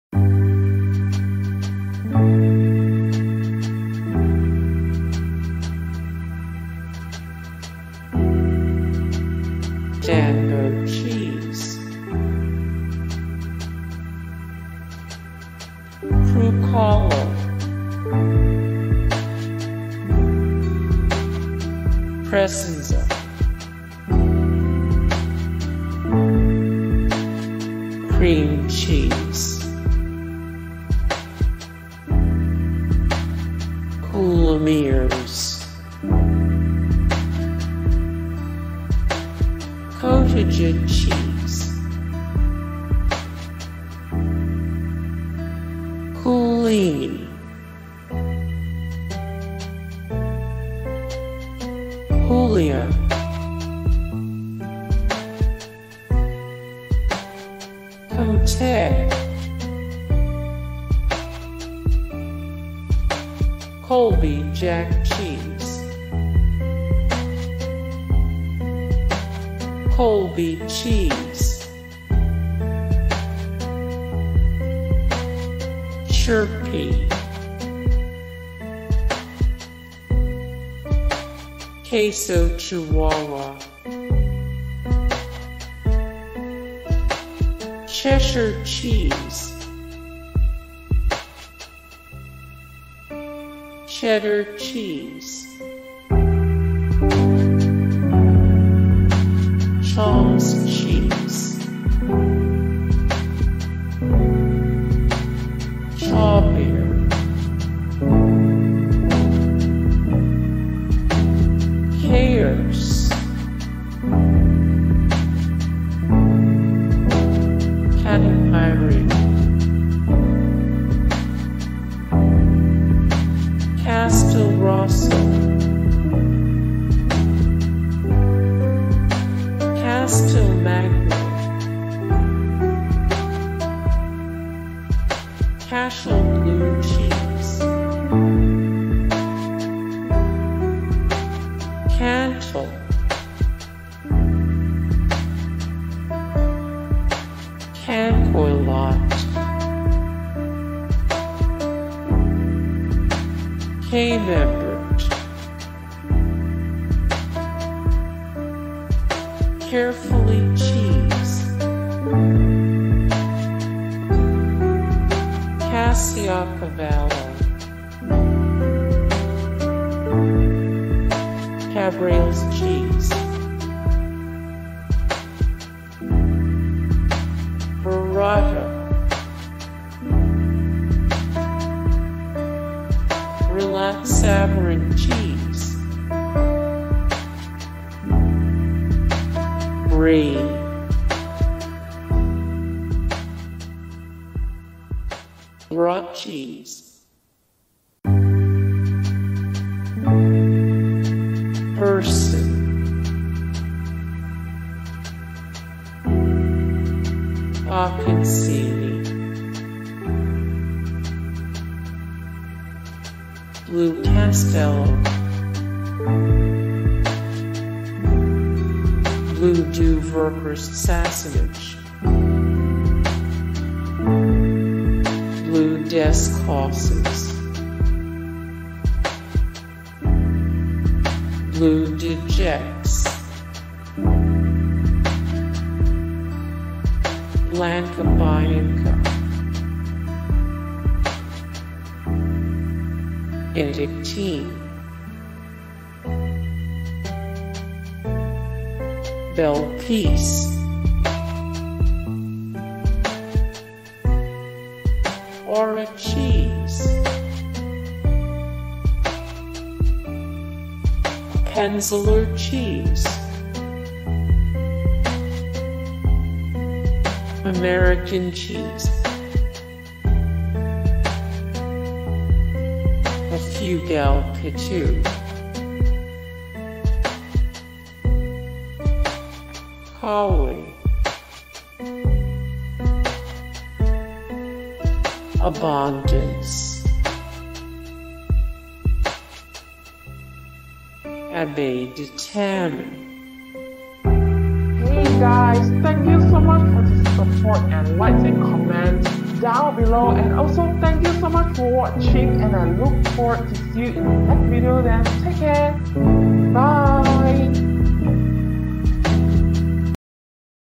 Cheese Made From Cow Milk 🌞 🌝
List of all cheese types/textures/ages made from cows milk, listed alphabetically with photos when available. This list includes cheese from all different regions/countries of origin and ingredients/recipes. For many of these cheeses you can click on their names to get even more information about them. If you're a cheese lover this list is the perfect way to find some cow milk cheese that you've never tried before. List includes items like Abondance and Ackawi cheese. This list answers the questions, "What is a list of cheeses that are made with cows milk?"...more
0:00 - Intro
0:00:08 - Danbo cheese
0:00:15 - Crucolo
0:00:22 - Crescenza
0:00:29 - Cream cheese
0:00:36 - Coulommiers
0:00:43 - Cotija cheese
0:00:50 - Cooleeney
0:00:57 - Coolea
0:01:04 - Comt
0:01:11 - Colby-Jack cheese
0:01:18 - Colby cheese
0:01:25 - Churpee
0:01:32 - Queso Chihuahua
0:01:39 - Cheshire cheese
0:01:46 - Cheddar cheese
0:01:53 - Chaumes cheese
0:02:00 - Chaubier
0:02:07 - Chaource
0:02:14 - Catupiry
0:02:21 - Castelrosso
0:02:28 - Castelmagno
0:02:35 - Cashel Blue cheese
0:02:42 - Cantal
0:02:49 - Cancoillotte
0:02:56 - Camembert
0:03:03 - Caerphilly cheese
0:03:10 - Caciocavallo
0:03:17 - Cabrales cheese
0:03:24 - Burrata
0:03:31 - Brillat-Savarin cheese
0:03:38 - Brie
0:03:45 - Bra cheese
0:03:52 - Boursin
0:03:59 - Bocconcini
0:04:06 - Blue Castello
0:04:13 - Bleu du Vercors-Sassenage
0:04:20 - Bleu des Causses
0:04:27 - Bleu de Gex
0:04:34 - Blanca Bianca
0:04:41 - Benedictine
0:04:48 - Bel Paese
0:04:55 - Aura cheese
0:05:02 - Appenzeller cheese
0:05:09 - American cheese
0:05:16 - Afuega'l pitu
0:05:23 - Ackawi
0:05:30 - Abondance
0:05:37 - Abbaye de Tamie